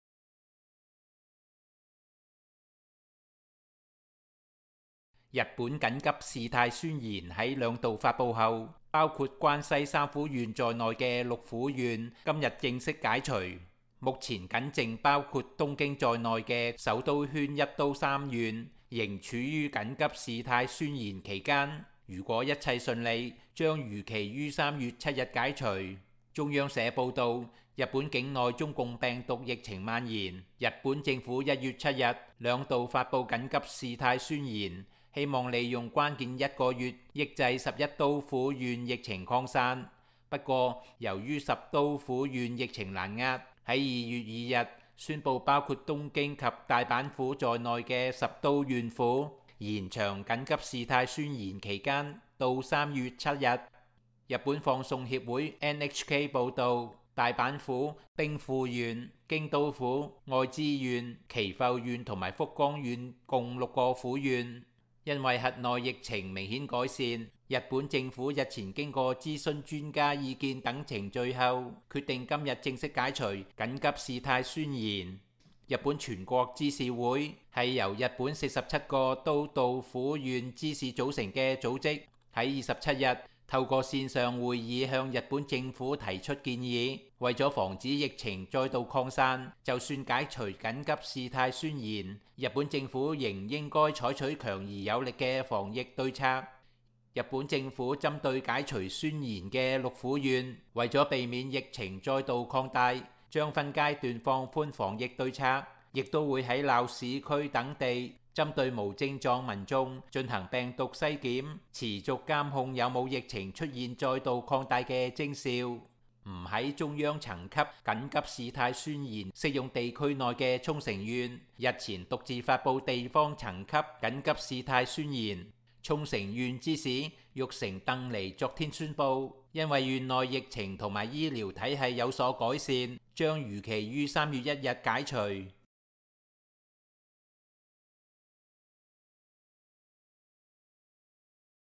日本解除緊急事態 東京等4地仍待觀察
日本「緊急事態宣言」2度發布後，包括關西3府縣在內的6府縣今天起正式解除，目前僅剩包括東京都在內的首都圈1都3縣仍處於「緊急事態宣言」期間，如果一切順利，將如期於3月7日解除。

中央社報導，日本境內中共病毒（COVID-19）疫情延燒，日本政府1月7日二度發布「緊急事態宣言」，希望利用「關鍵一個月」抑制11都府縣疫情擴散，不過，由於10都府縣疫情難遏，2月2日宣布包括東京都及大阪府在內的10都府縣，延長「緊急事態宣言」期間到3月7日。

日本放送協會（NHK）報導，大阪府、兵庫縣、京都府、愛知縣、岐阜縣及福岡縣共6府縣，因為轄內疫情明顯改善，日本政府日前經諮詢專家意見等程序後，決定今天正式解除「緊急事態宣言」。

日本全國知事會（由日本47個都道府縣知事組成的組織）27日透過線上會議向日本政府提出建言，為防止疫情再度擴散，就算解除「緊急事態宣言」，日本政府仍應該採取強而有力的防疫對策。

日本政府針對解除宣言的6府縣，為避免疫情再度擴大，將分階段放寬防疫對策，也會在鬧區等地針對無症狀民眾進行病毒篩檢，持續監控有無出現疫情再度擴大的徵兆。

不在中央層級「緊急事態宣言」適用地區內的沖繩縣，日前獨自發布地方層級「緊急事態宣言」，沖繩縣知事玉城鄧尼昨天宣布，因為縣內疫情及醫療體系有所改善，將如期於3月1日解除。